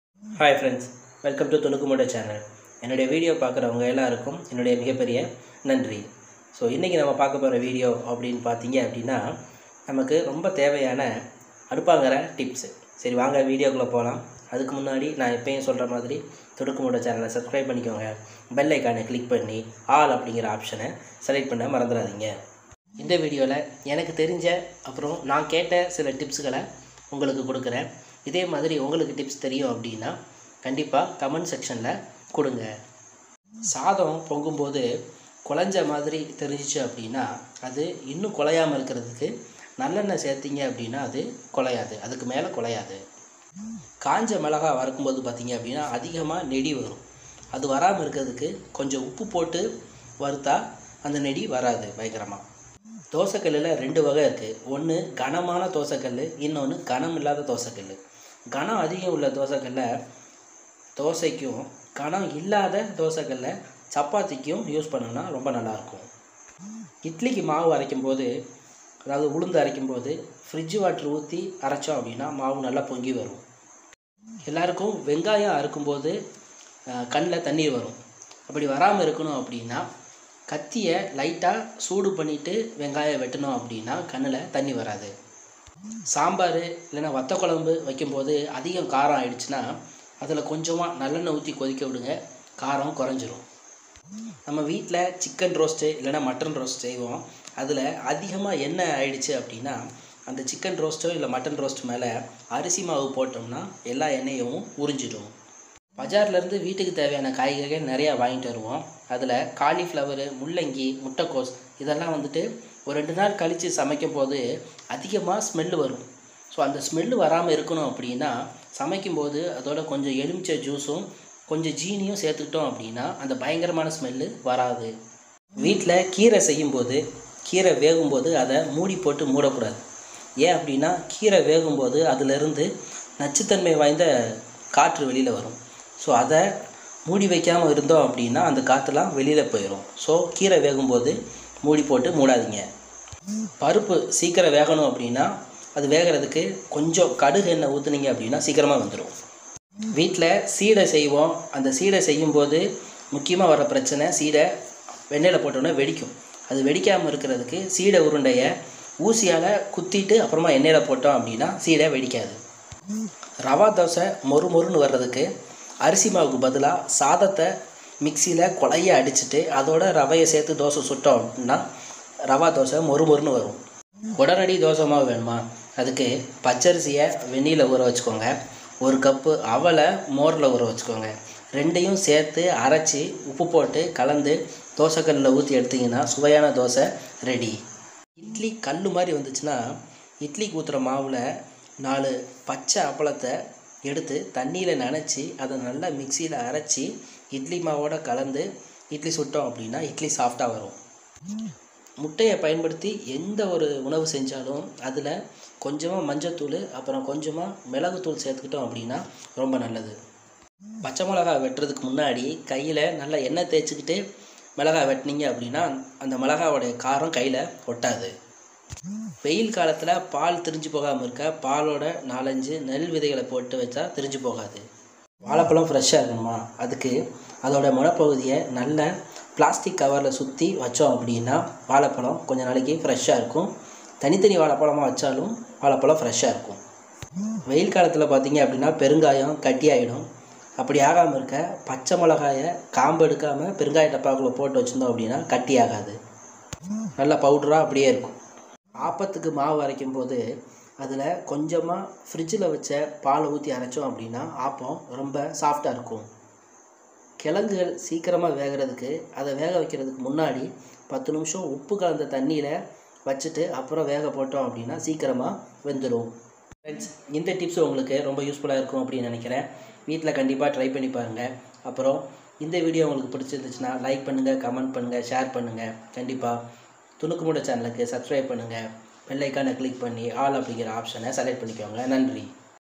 Useful Samayal tips | Tamil | Thunuku mootai
This video gives you very much useful kitchen tips. You can try this at your home. If you any know tips please type in comments box.